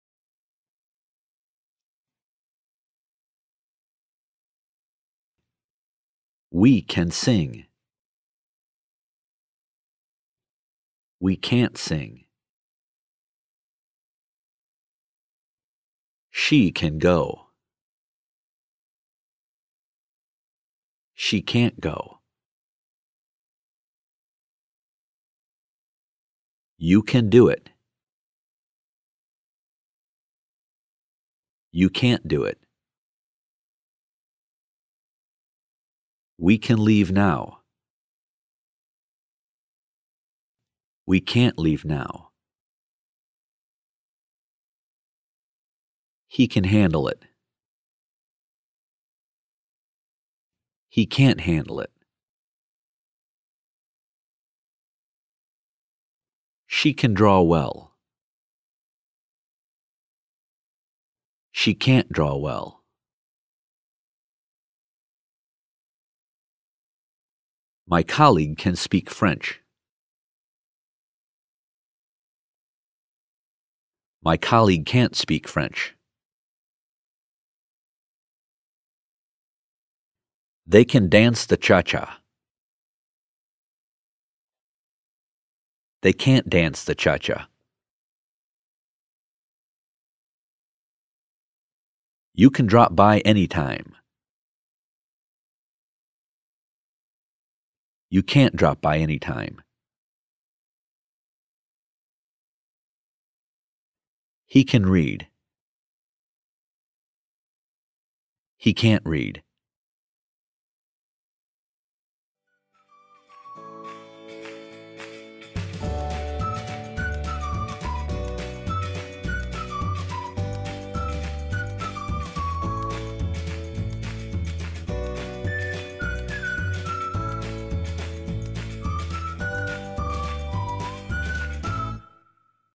CAN vs CAN'T PRONUNCIATION - LEARN HOW TO PRONOUNCE "CAN" vs "CAN'T" LIKE A NATIVE ENGLISH SPEAKER
In this lesson, you will learn how to pronounce "can" and "can't" like a native English speaker. Keep in mind that you cannot drop the "T" sound in words like "can't," "wait," etc.  I hope this lesson helps you understand how to make the correct sound for each word, and you're able to tell the difference between them right way.

En esta lección aprenderás a pronunciar "can" y "can't" como un hablante nativo de inglés. Recuerda que no puedes dejar de pronunciar la "t" en palabras como "can't," "wait," etc. Espero que esta leccion te ayude a entender la correcta pronunciación para cada palabra y seas capaz de notar la diferencia entre ellas al instante.